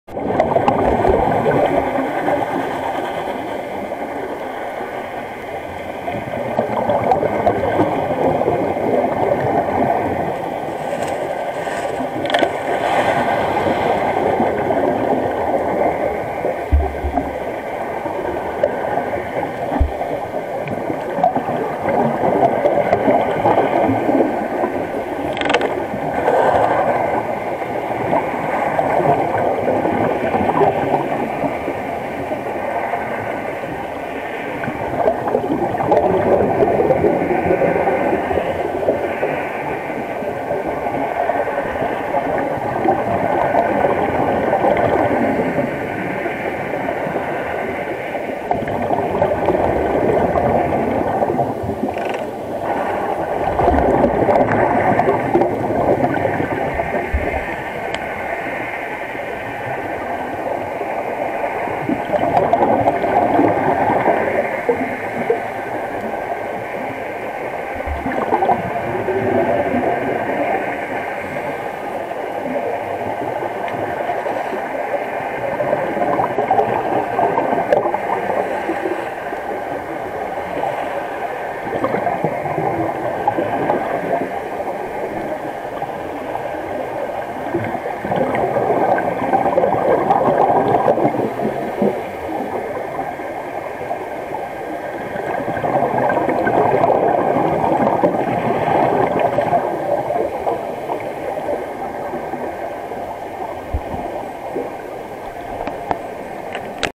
Elphistone - Marsa Alam 2019-11-28, druhý ponor III, žraloci dlouhoploutví. Oceanic Whitetip Sharks
Potápění na Elphistone - Marsa Alam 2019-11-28, druhý ponor III- žraloci dlouhoploutví tu údajně byli tři, já jsem však viděl pouze dva.
Max. hloubka ponoru 31 metrů, délka ponoru: 57 min., minimální teplota 26°C